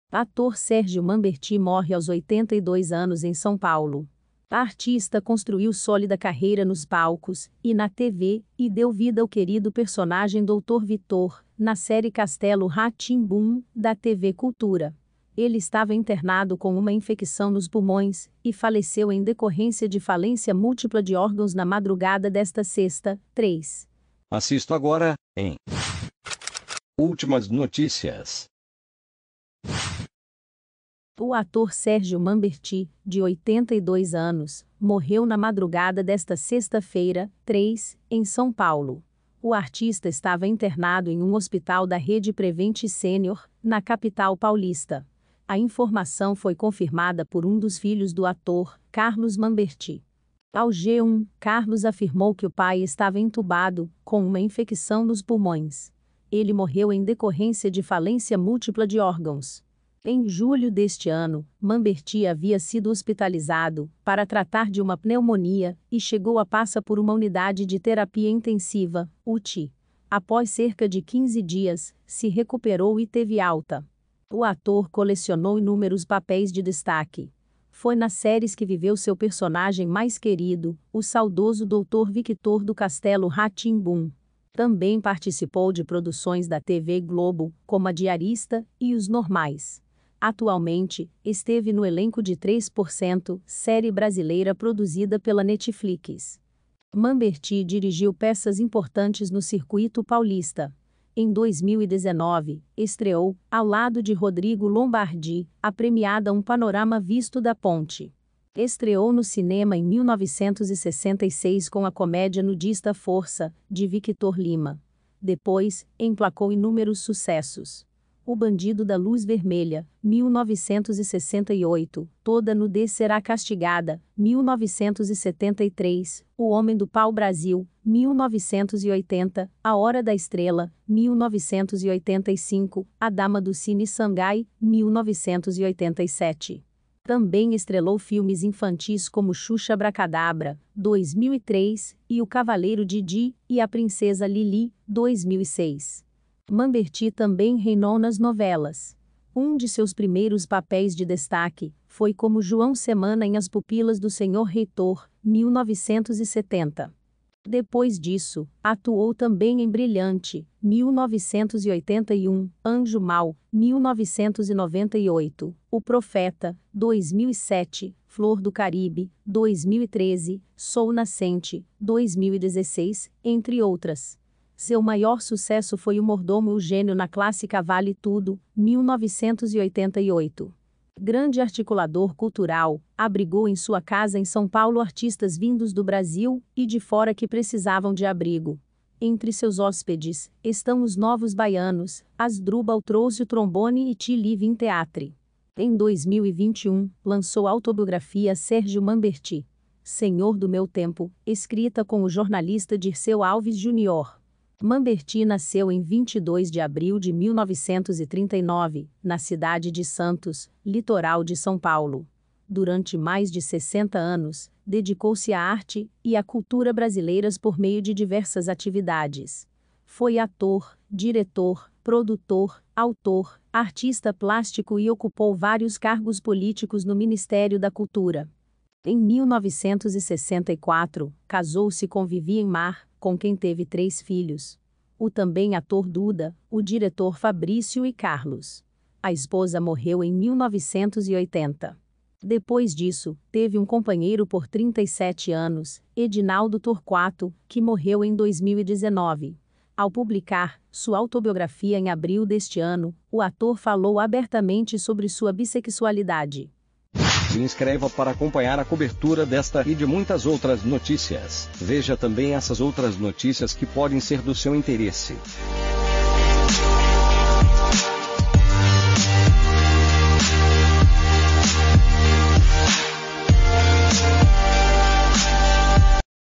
Ator Sérgio Mamberti morre aos 82 anos em SP
Artista construiu sólida carreira nos palcos e na TV, e deu vida ao querido personagem Doutor Vitor, na série 'Castelo Rá-Tim-Bum', da TV Cultura. Ele estava internado com uma infecção nos pulmões e faleceu em decorrência de falência múltipla de órgãos na madrugada desta sexta (3).

Acessado em: 3 de setembro de 2021 às 08:12